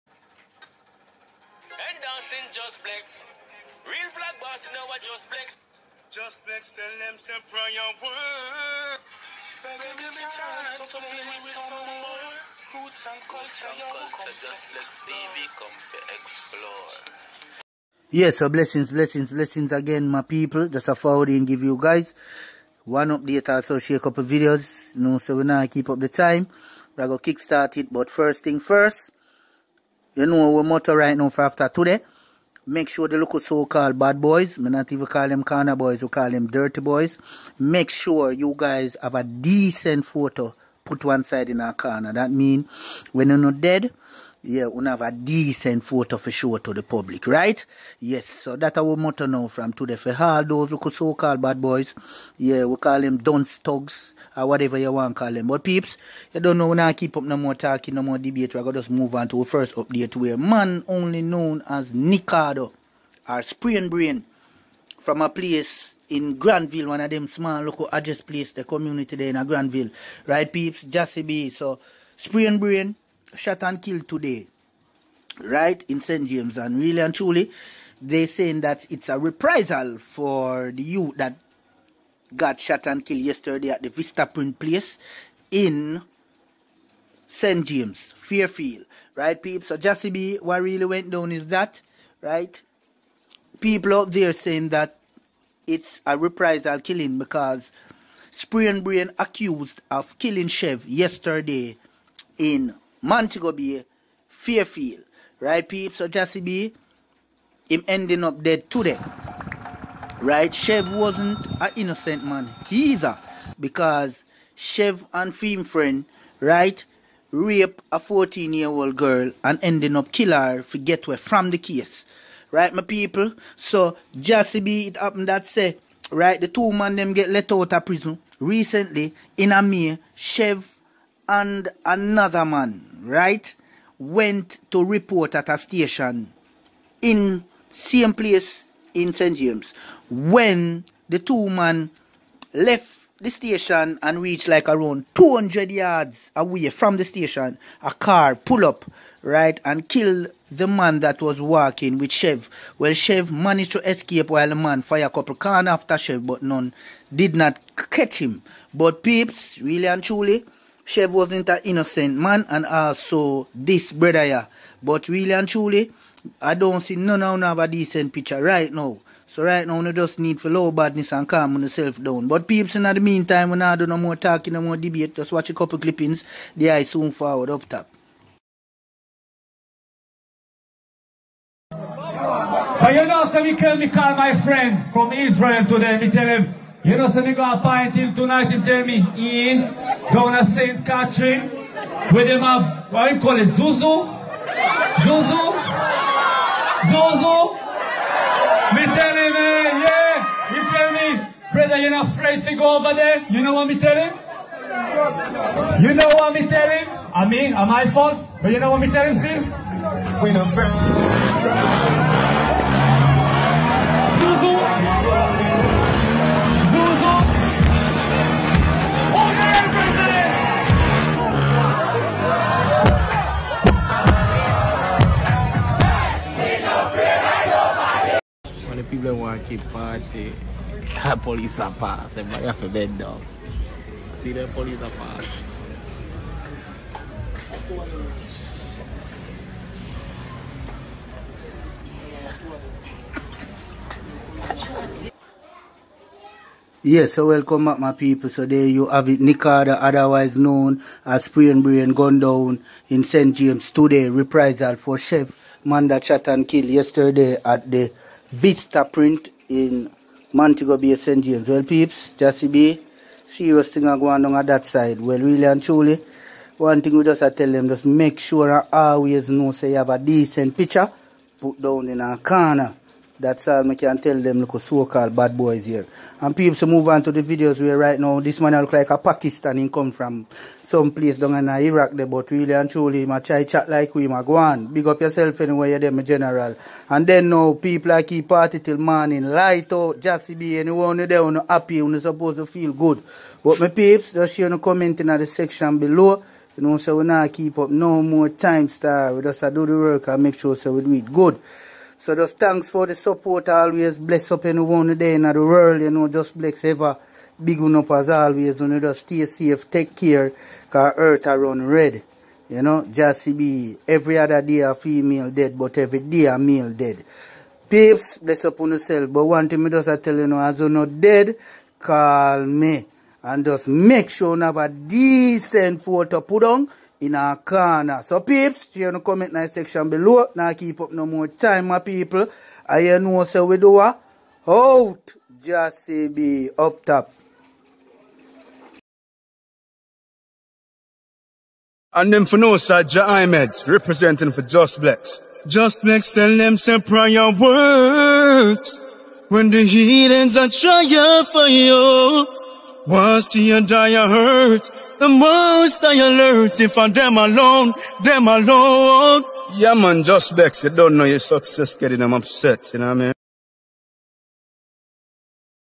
Sprain Brain K!(( Man Yesterday And D3@D Today St Elizabeth/+Alot More Aug 12 2021
Sprain Brain K!(( Man Yesterday And D3@D Today St James+Alot More Aug 12 2021